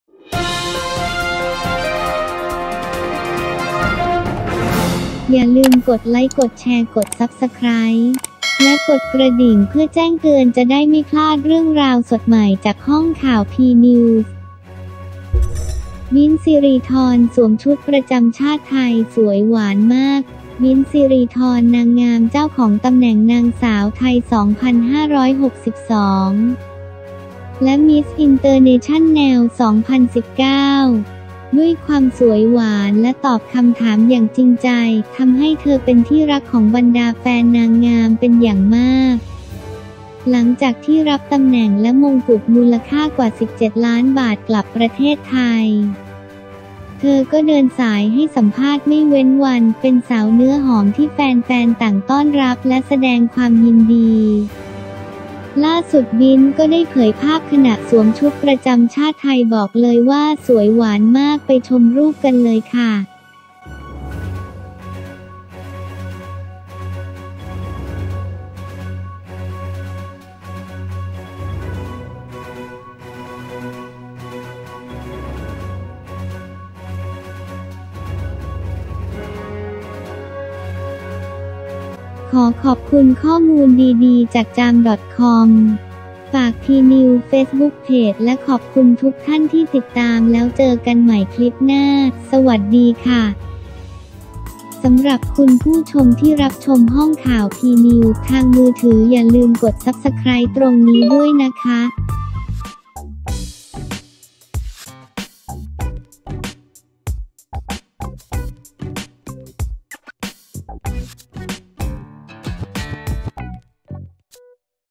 ห้องข่าวพีนิวส์ | บิ๊นท์ สิรีธร มิสอินเตอร์เนชั่นแนล 2019 สวมชุดประจำชาติไทย สวยหวานมาก
ขอขอบคุณข้อมูลดีดีจาก จาม ดอทคอม

ฝากพีนิวเฟซบุ๊กเพจและขอบคุณทุกท่านที่ติดตามแล้วเจอกันใหม่คลิปหน้า สวัสดีค่ะ